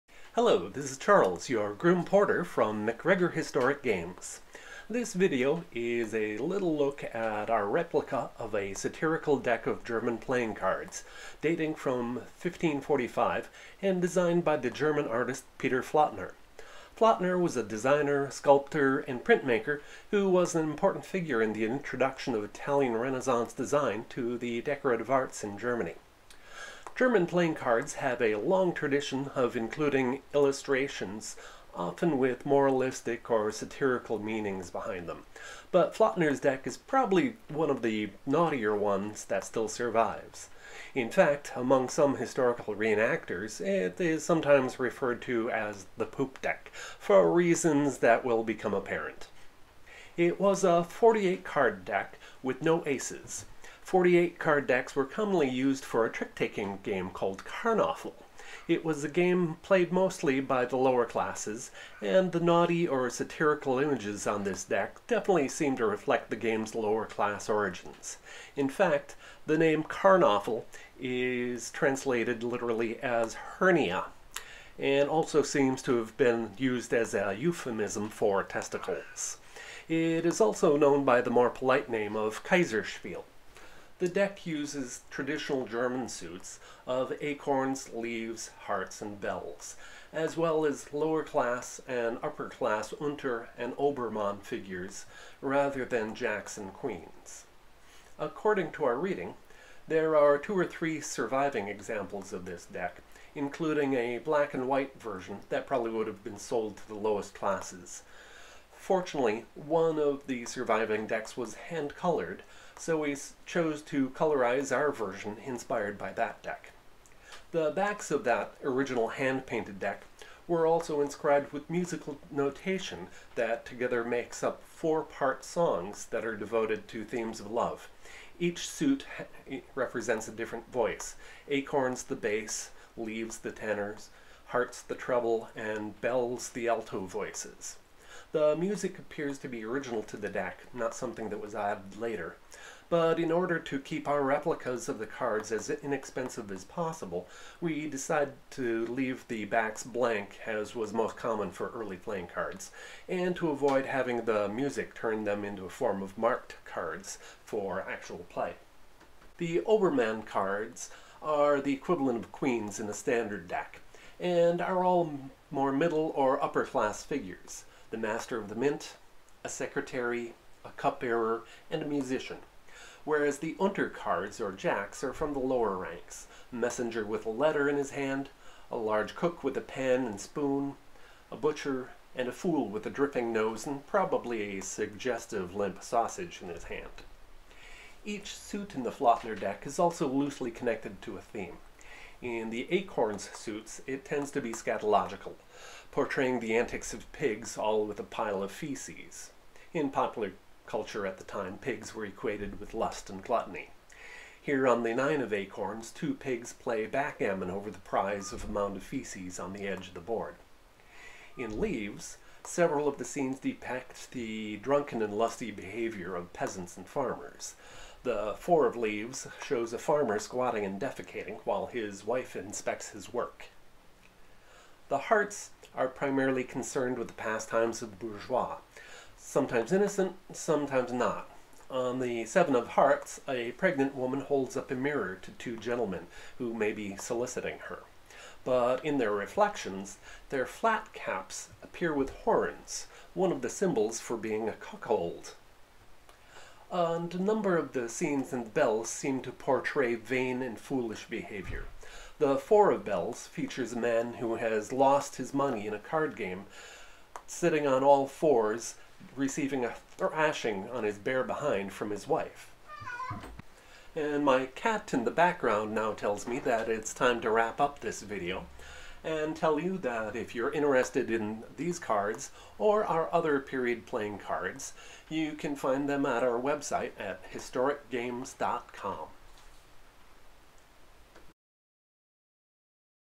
16th Century Satirical German Playing Cards
A quick look at the symbolism of the illustrations of this historic deck of cards